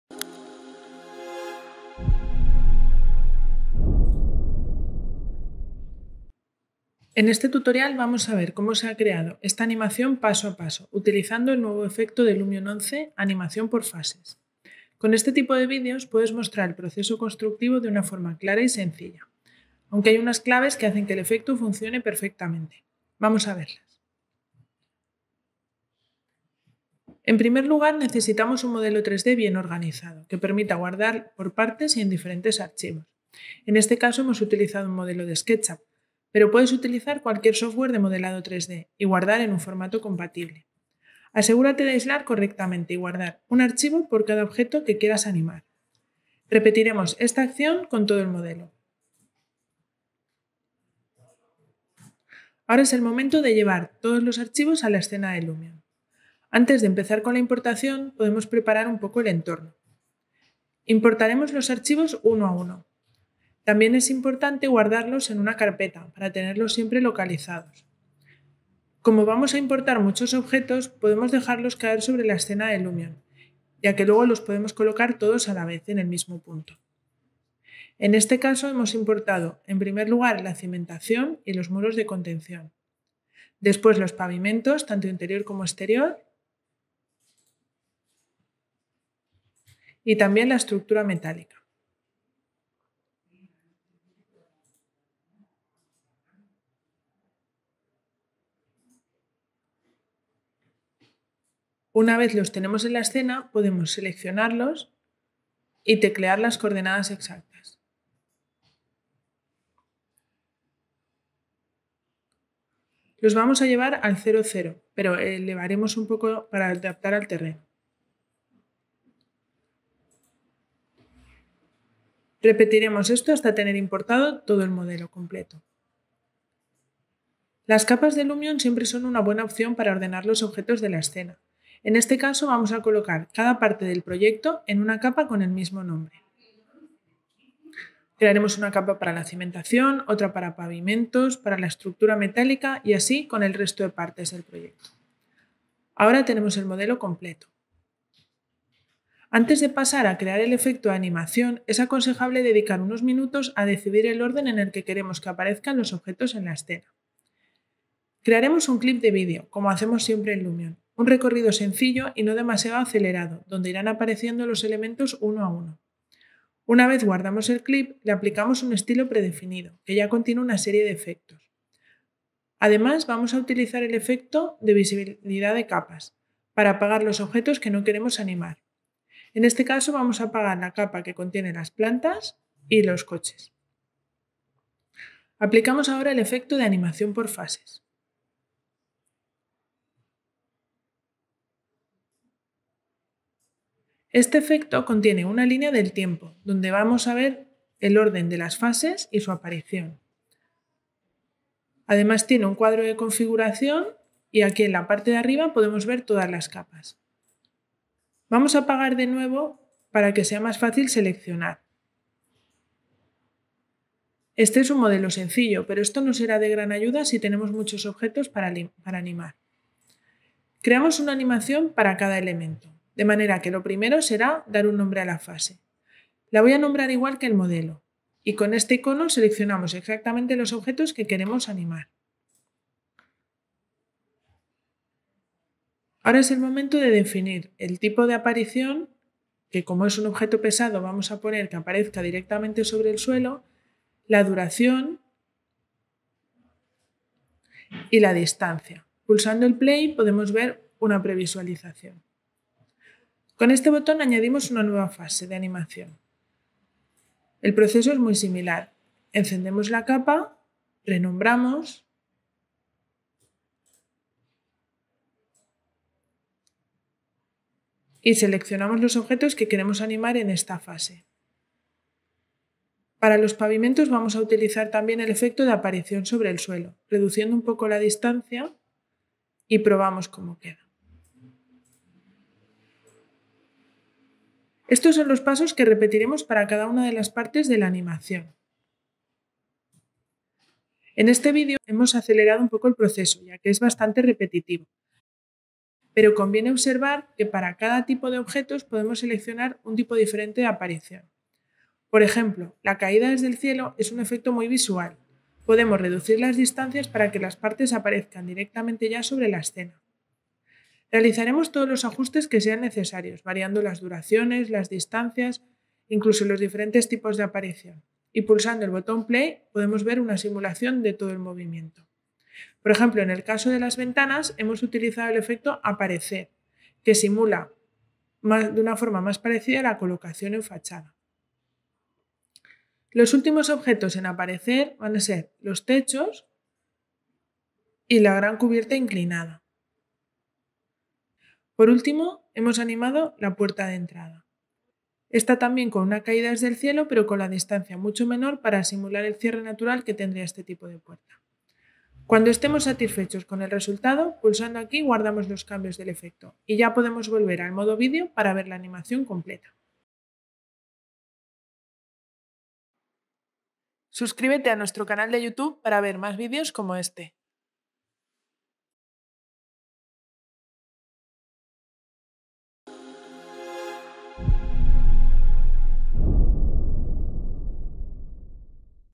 Animación por fases: Tutorial básico - Lumion
En este tutorial básico te explicamos cómo funciona el efecto Animación por fases, disponible en Lumion. Con este tutorial vas a poder aprender las nociones más sencillas y cómo aplicar este efecto en tu proyecto.

Créditos del modelo 3D:  Brightman Designs